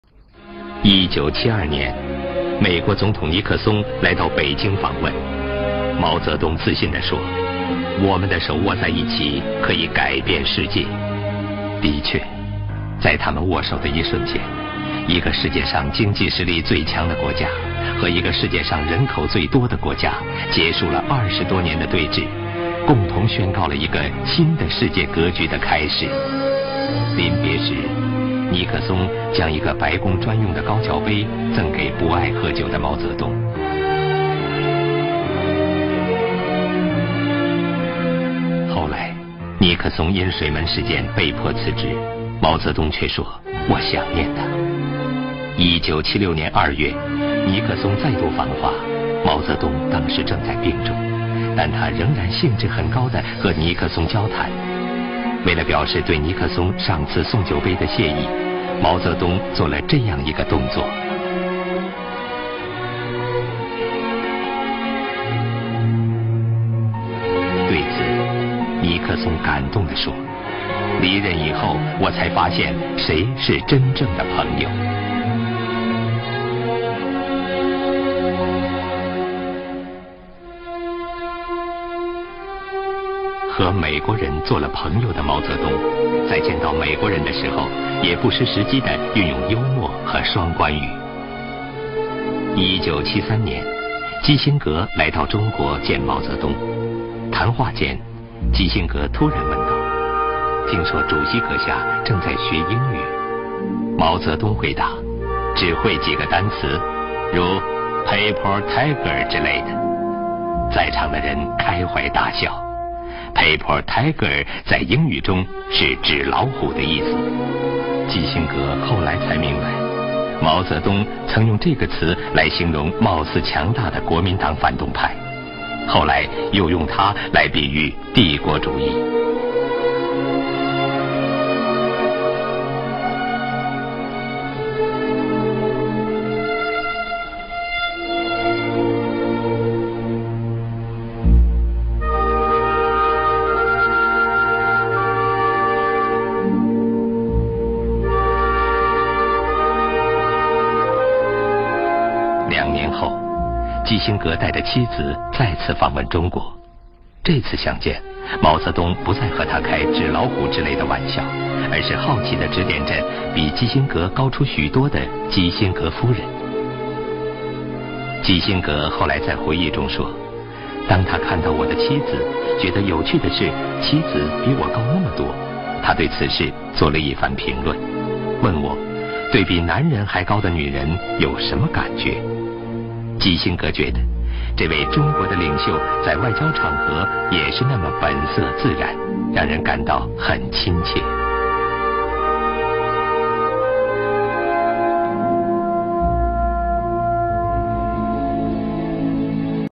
1972 PRESIDENT NIXON MET CHAIRMAN MAO 毛主席会见了尼克松总统
毛主席会见了尼克松总统 Mao Zedong met Henry Kissinger several times, Richard Nixon, and Gerald Ford.